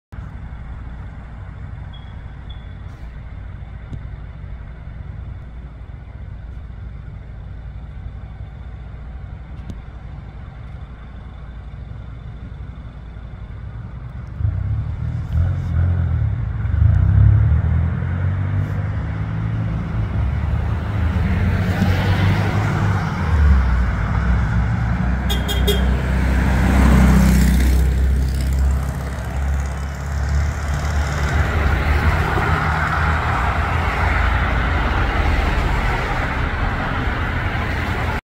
Herbie the Love Bug passes through Olyphant, PA, October 8, 2022
Yes it's almost certainly a replica, but you never know.